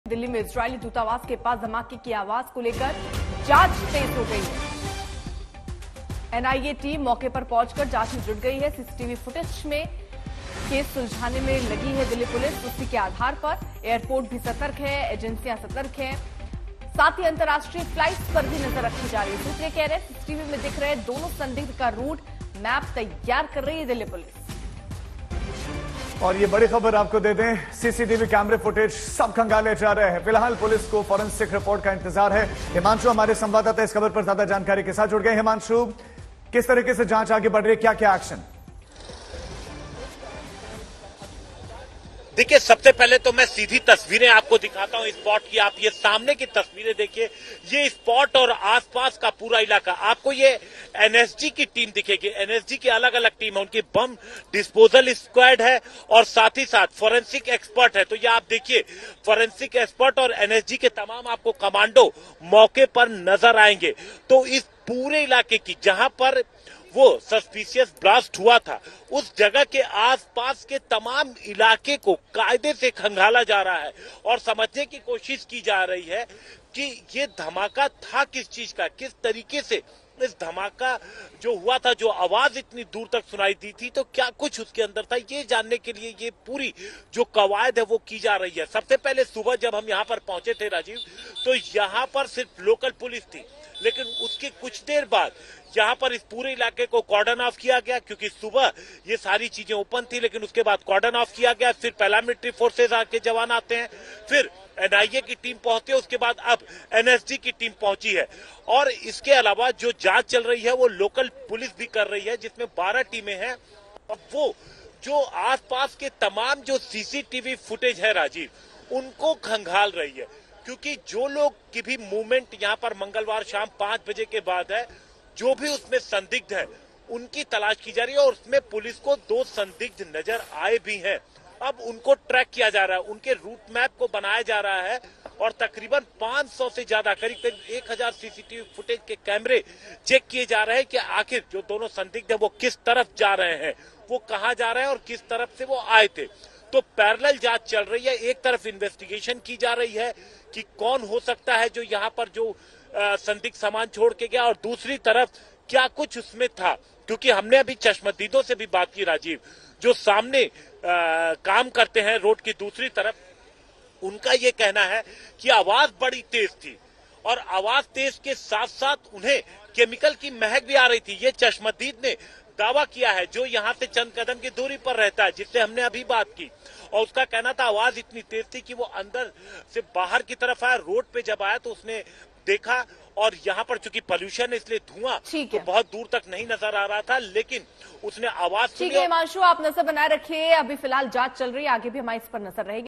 Israel Embassy Attack: Delhi में इजरायली दूतावास के पास धमाके की जांच तेज, पहुंची NIA और NSG की टीम
दिल्ली (Delhi) में इजरायली दूतावास के पास धमाके की आवाज को लेकर जांच चेज हो गई हैं- NIA टीम मौके पर पहुंचकर जांच में जुट गई है. सीसीटीवी फुटेज से केस सुलझाने में लगी है दिल्ली पुलिस. एयरपोर्ट पर भी सतर्क है एजेंसी -साथ ही अंतरराष्ट्रीय फ्लाइट्स पर नजर रखी जा रही है. सूत्रों के मुताबिक सीसीटीवी में दिख रहे दोनो संदिग्ध का रूट मैप तैयार कर रही है दिल्ली पुलिस. इसके लिए आसपास के 1000 से ज्यादा सीसीटीवी कैमरों के फुटेज की जांच में जुटी हुई है पुलिस. देखें ये वीडियो.